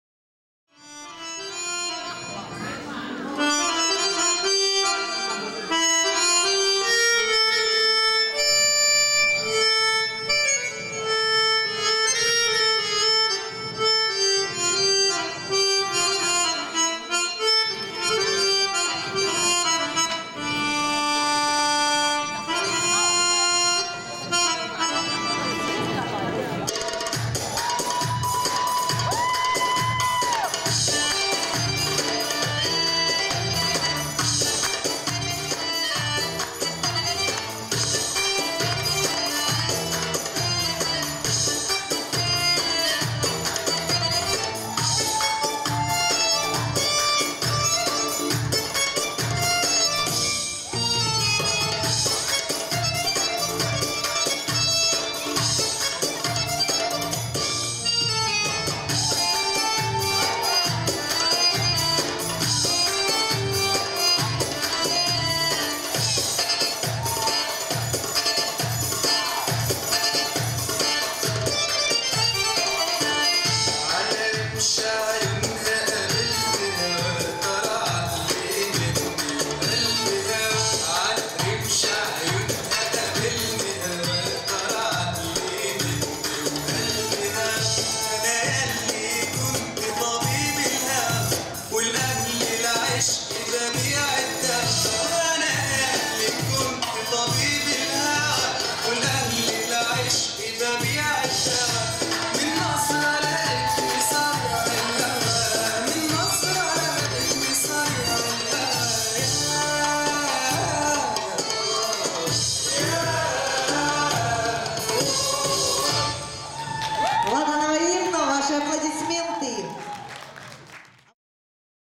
Inna Ladanova ⊰⊱ Fiesta Dance '13.
Инна Ладанова на Пятом Всеукраинском конкурсе современного и восточного танца "FIESTA DANCE" во Дворце Молодежи «Юность» г. Донецк 16.11.13. (Взрослые начинающие) 44,80 .